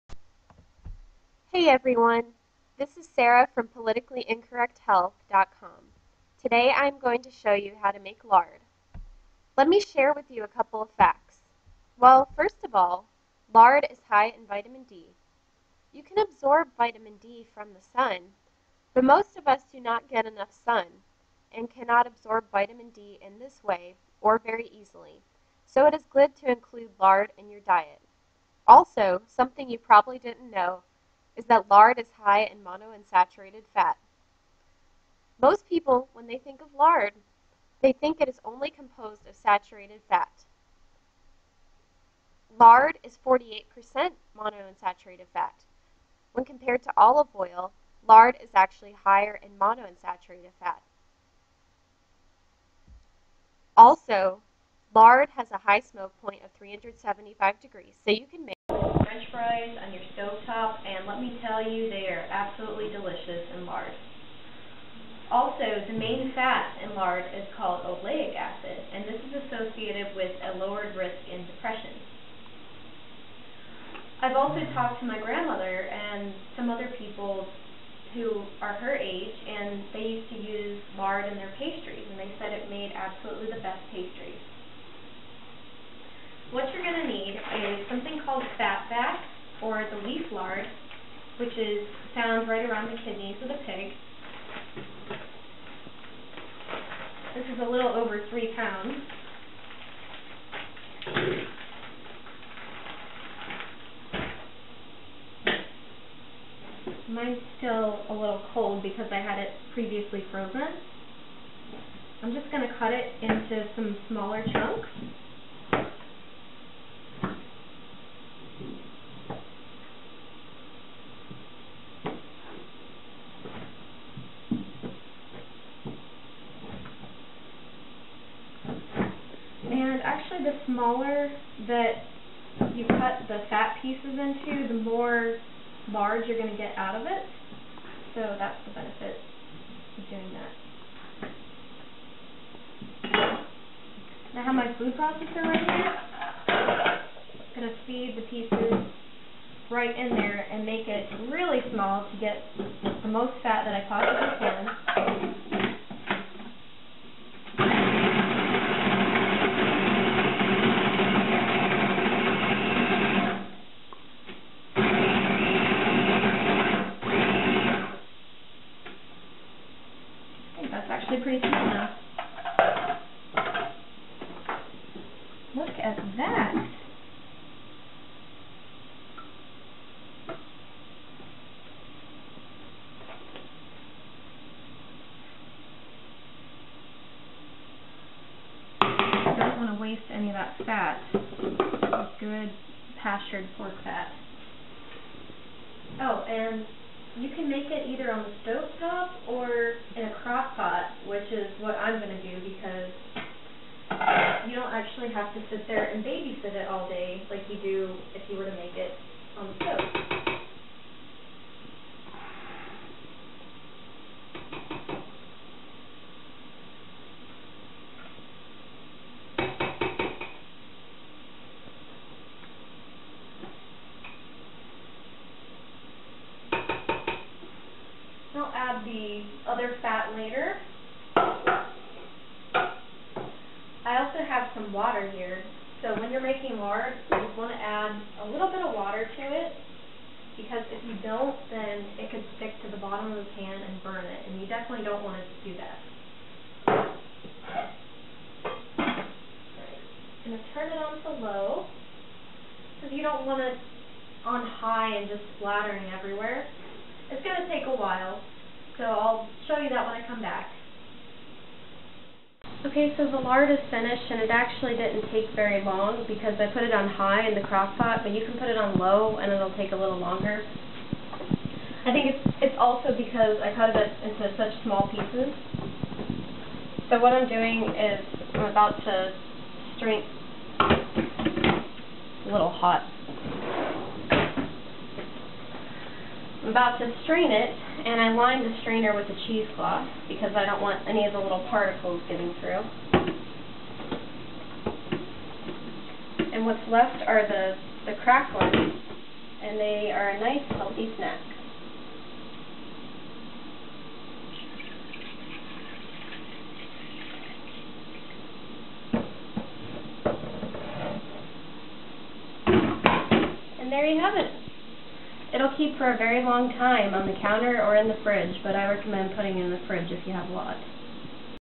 How to Make Lard
How to make lard in your slow cooker!